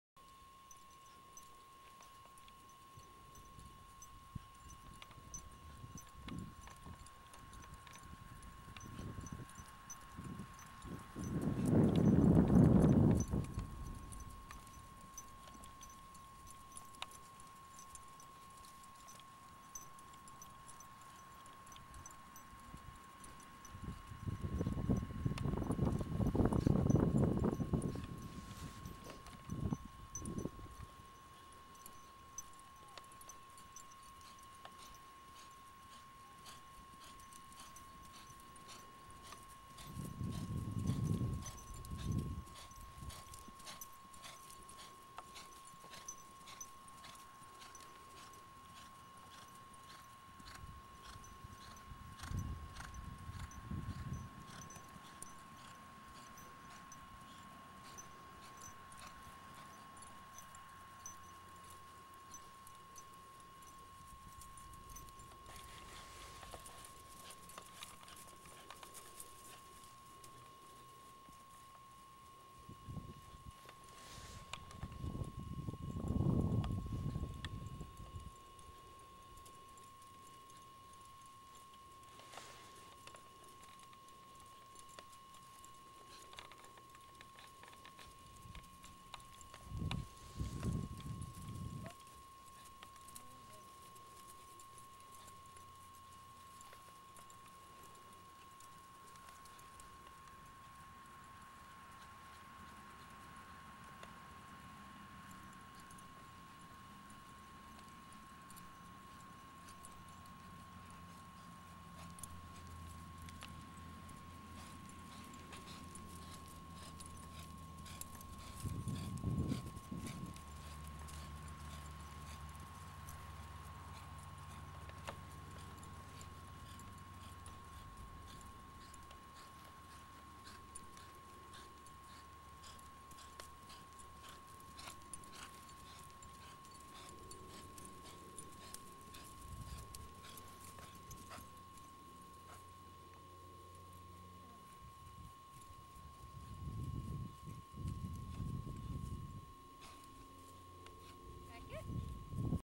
Tori flat work
Tori doing some trotting and cantering on a circle on her 14th ride. March 13, 2009.